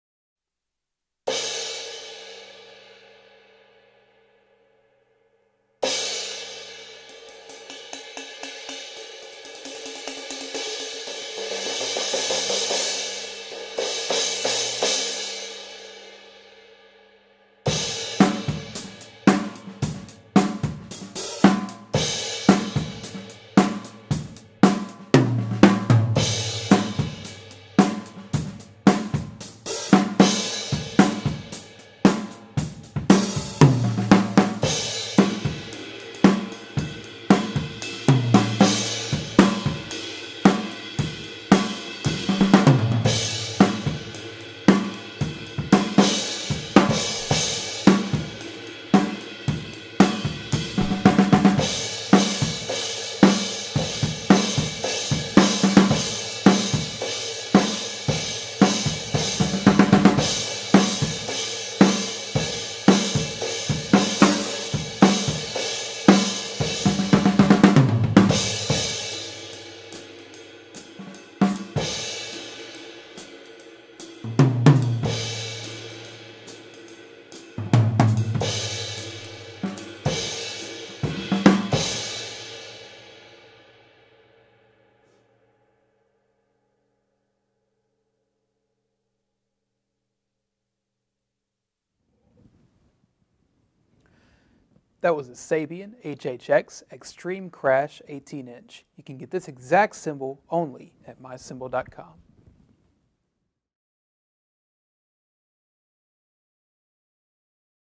Sabian 18" HHX X-Treme Crash Cymbal - 1314g (11892XN-1062216V)
*SOLD* Sabian 18" HHX X-Treme Crash Cymbal.

Other gear used in this video:
• Cymbals (From drummers L to R): Sabian 15" Artisan Hi Hat Cymbals, Sabian 20" Artisan Light Ride Cymbal